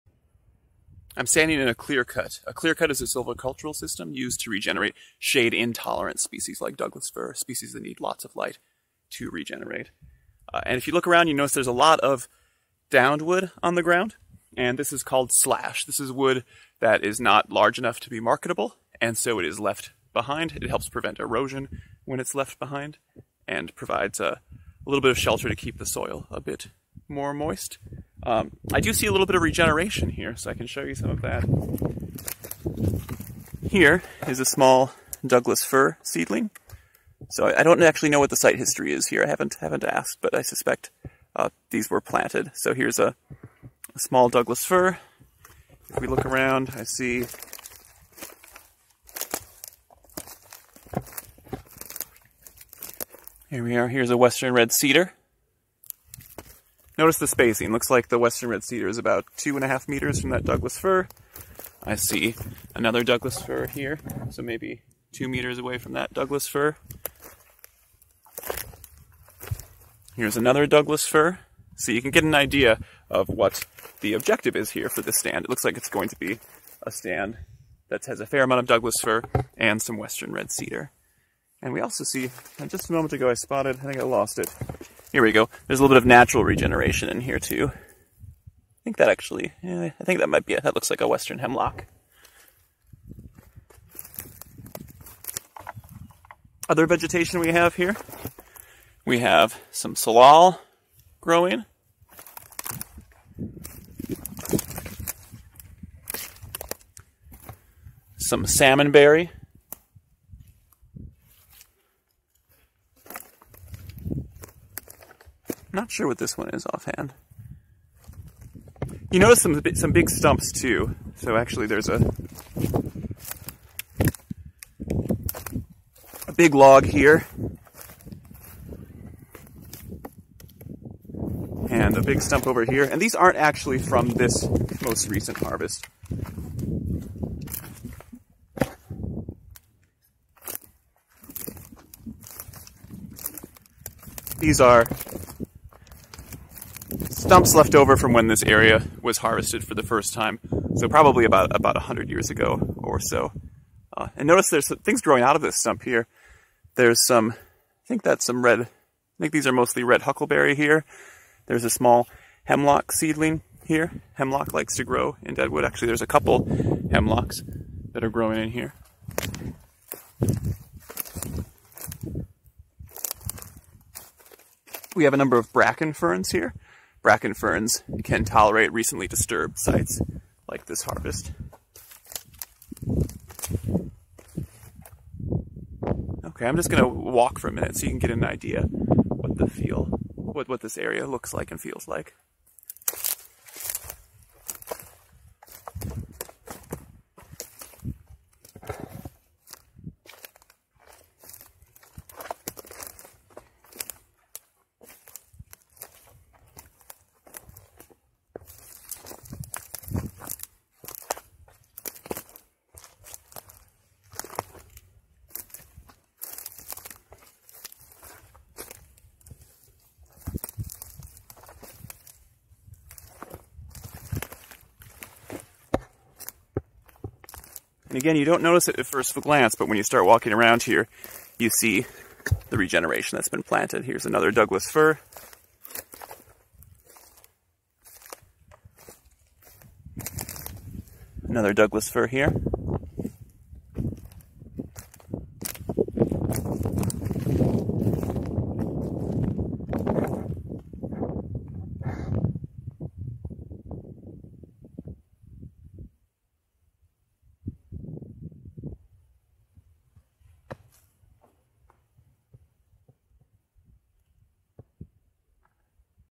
A Brief Walkthrough of a Recent Clearcut (360° Video)
A 360-degree walkthrough of a recently-harvested clearcut at Malcolm Knapp Research Forest.

Filmed by Patrick Culbert, University of British Columbia - Faculty of Forestry